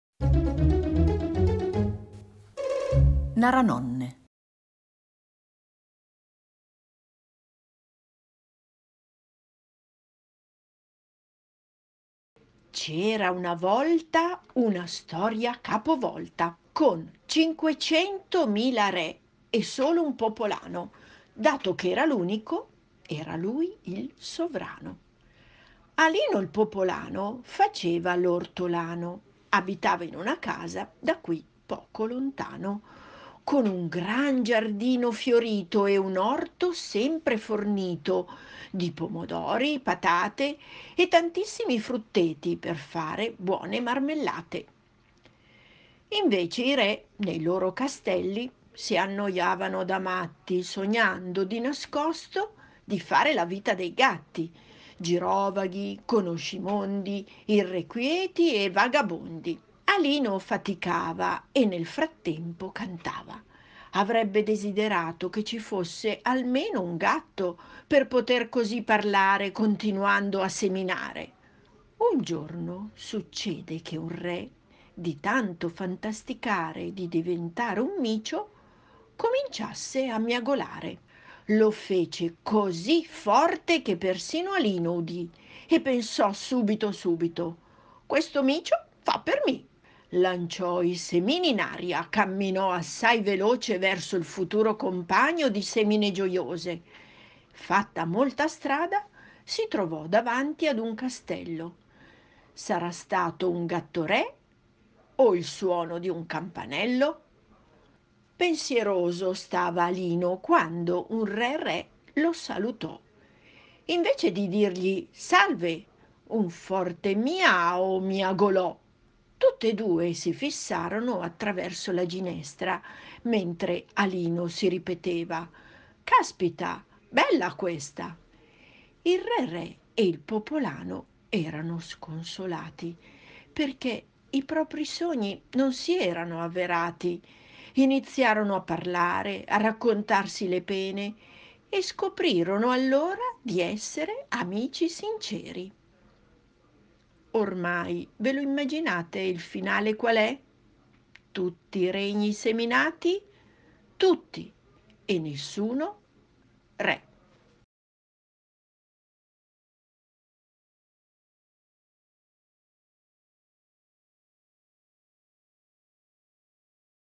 Alino il Popolano, di Fernanda Menéndez
Storie in viva voce 2021 - Speciale 1° maggio.
Per tutti i bambini che amano leggere, ascoltare e osservare!
Legge Laura